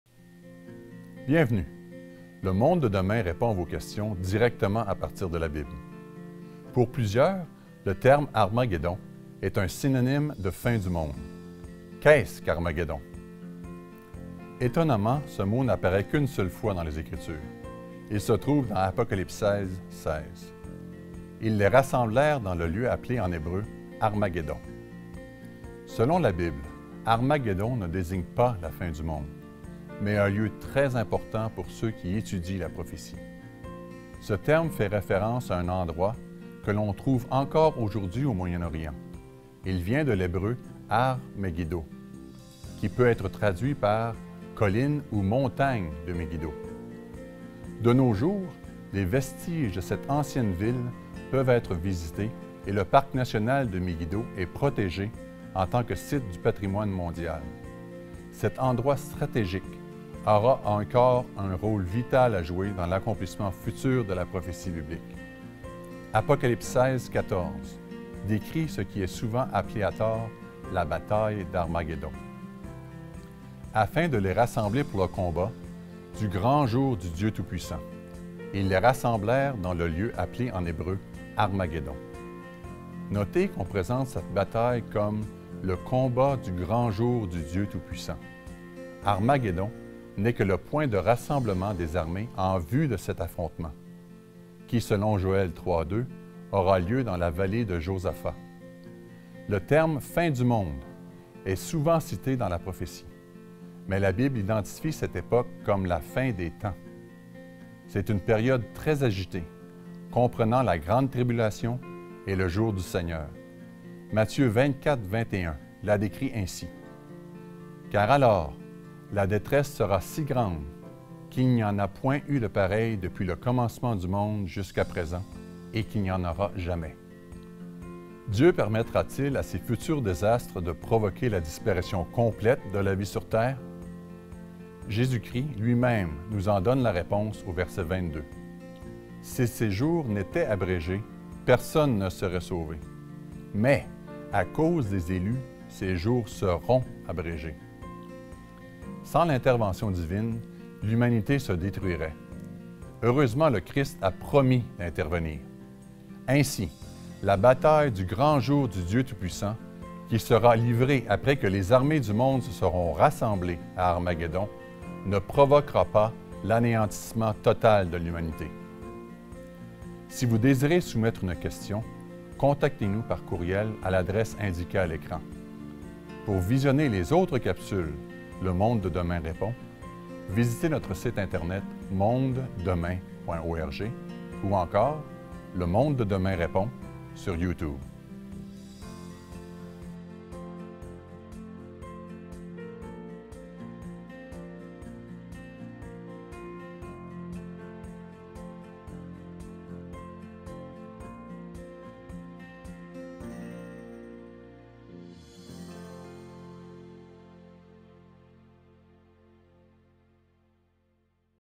Qu’est-ce qu’Harmaguédon?
Pour beaucoup, le terme "Harmaguédon" est synonyme de "fin du monde". Qu'est-ce qu’Harmaguédon?

Étonnamment, ce mot n’apparaît qu’une seule fois dans les Écritures: "Ils les rassemblèrent dans le lieu appelé en hébreu Harmaguédon" (Apocalypse 16 :16).

Selon la Bible, Harmaguédon ne désigne pas la fin du monde, mais un lieu très important pour ceux qui étudient la prophétie. Ce terme fait référence à un endroit que l’on trouve encore aujourd'hui au Moyen-Orient. Il vient de l’hébreu « har Megiddo », qui peut être traduit par "colline ou montagne de Megiddo".

De nos jours, les vestiges de cette ancienne ville peuvent être visités et le parc national de Megiddo est protégé en tant que site du patrimoine mondial. Cet endroit stratégique aura encore un rôle vital à jouer dans l'accomplissement futur de la prophétie biblique. Apocalypse 16 :14 décrit ce qui est souvent appelé "la bataille d'Harmaguédon".